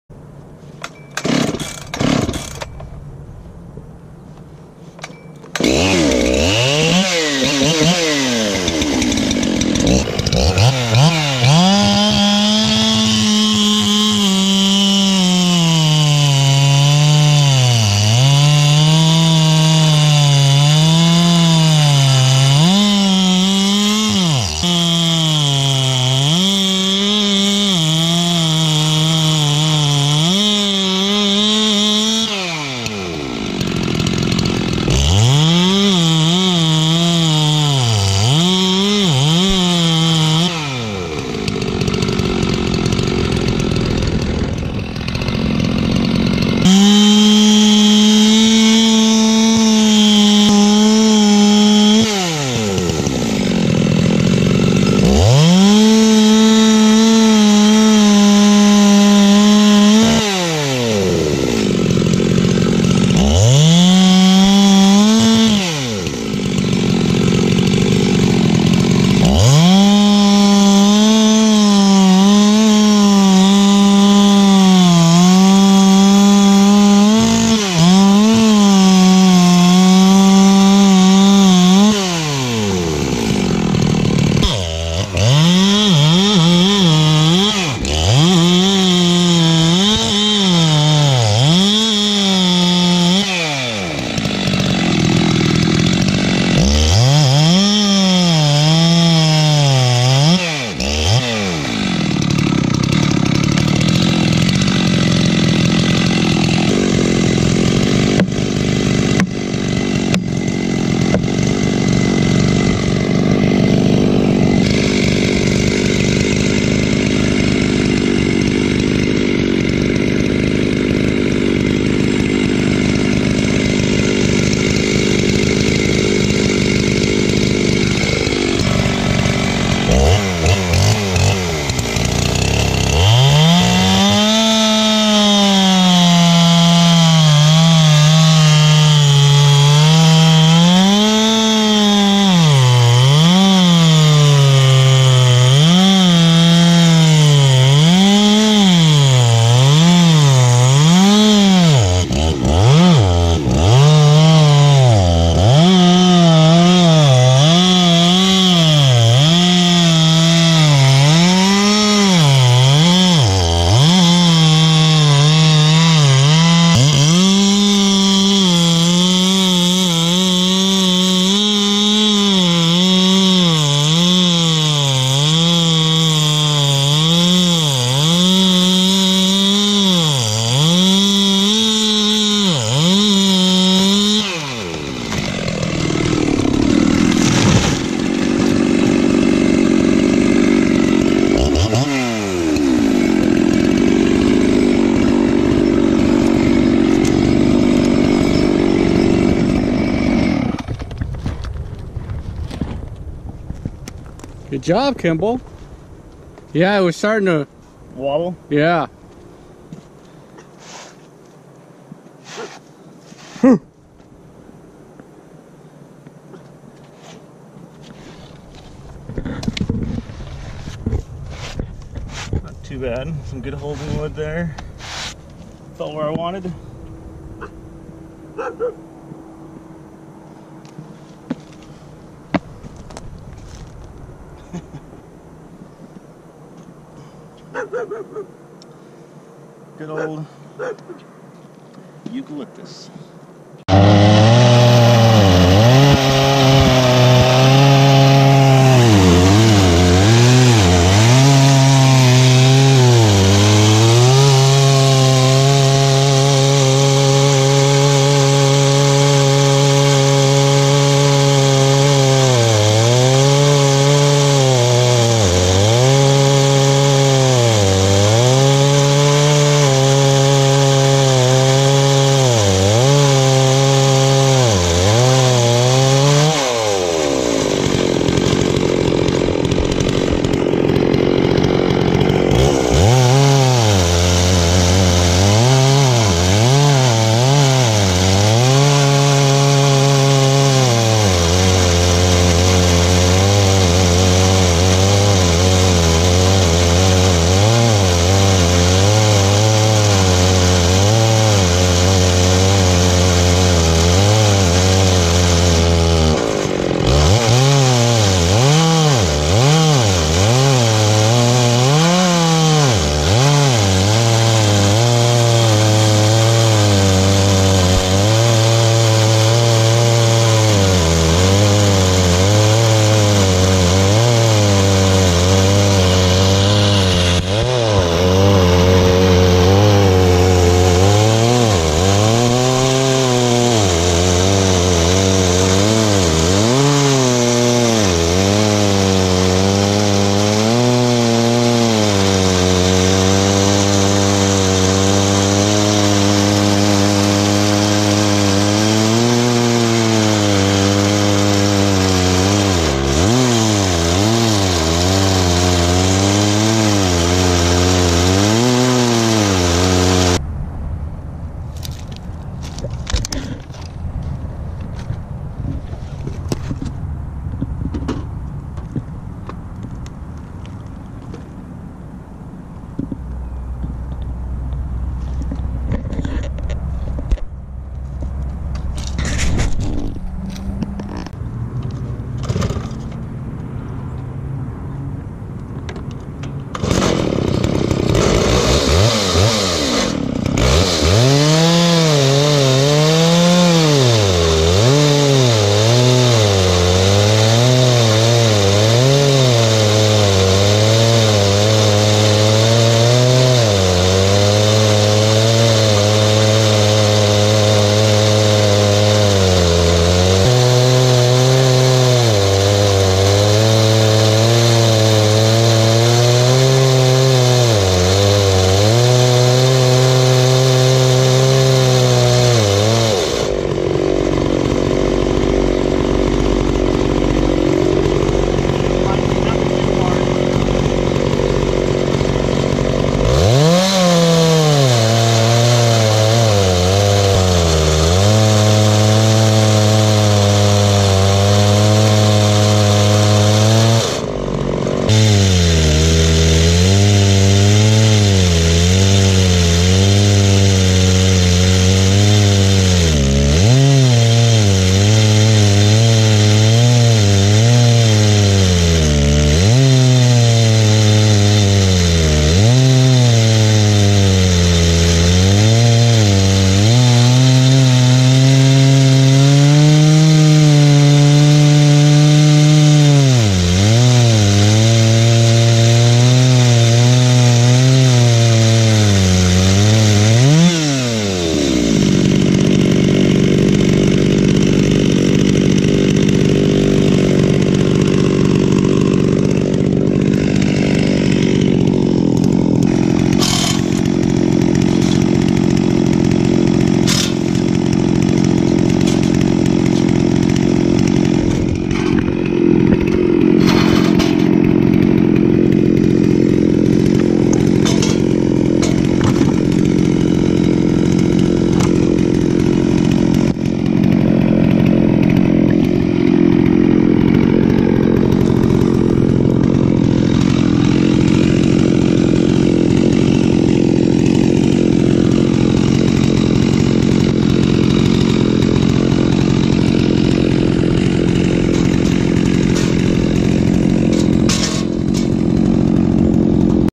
Cutting down and milling Red Gum Eucalyptus wood
Cutting down a dead standing red gum eucalyptus tree in Arizona. Native to Australia these eucalyptus were brought to Arizona in the mid to late 1800s where they thrive in the desert heat.

I used a modified Makita/Dolmar 7900 79cc saw with a 24" bar and chain to cut it down and a Chinese clone MS660 with a 42" bar and a PantherPros mill to slab it.

Red Gum is extremely dense and hard when dry.  Many people might say that my chains are dull but I can assure you that they are sharp.